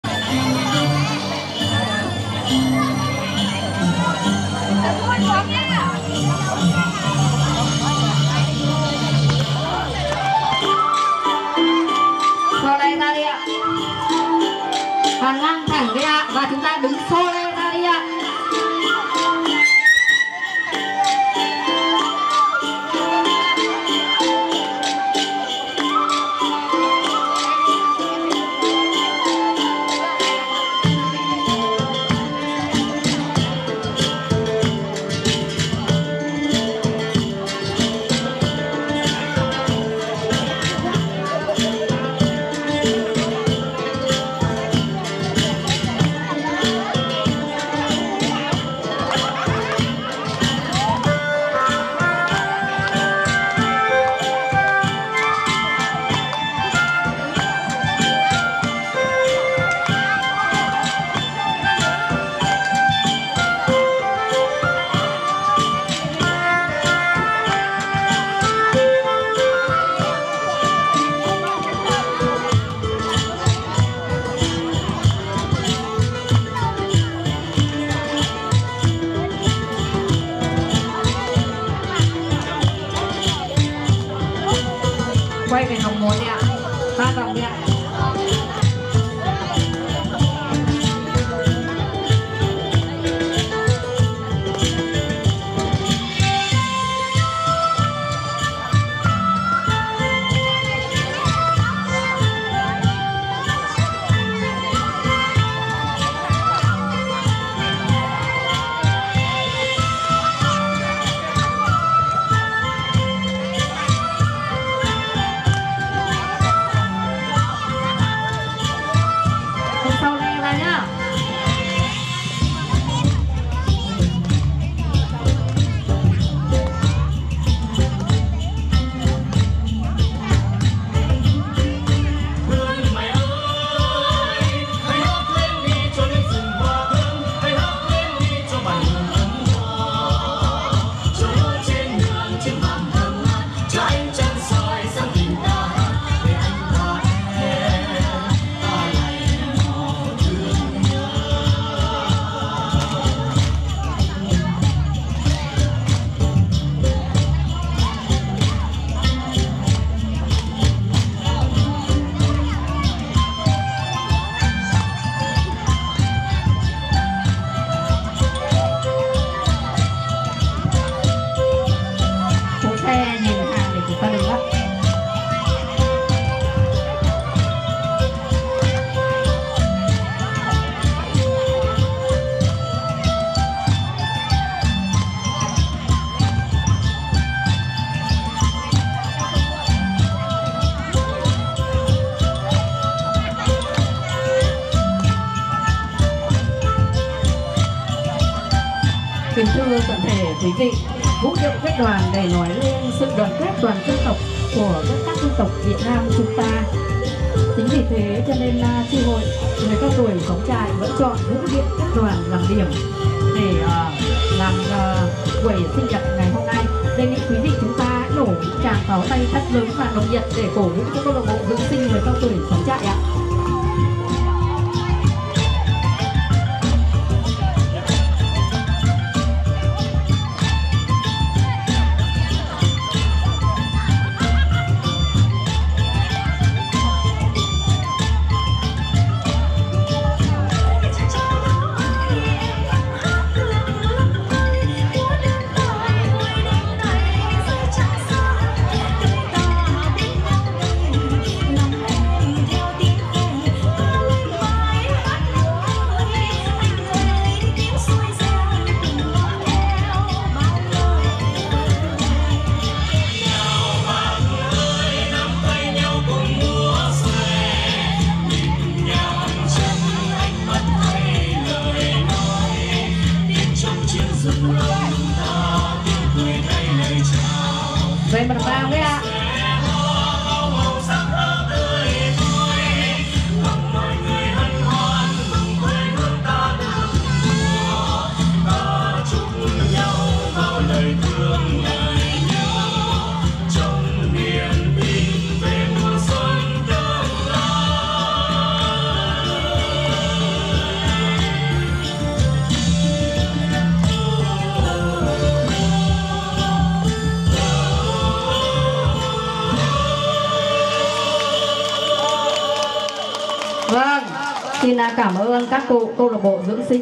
lễ kỷ niệm một năm. ngày thành lập CLB dưỡng sinh người cao tuổi xóm trại .ck pt.
xương thịnh ngày 14/04/2024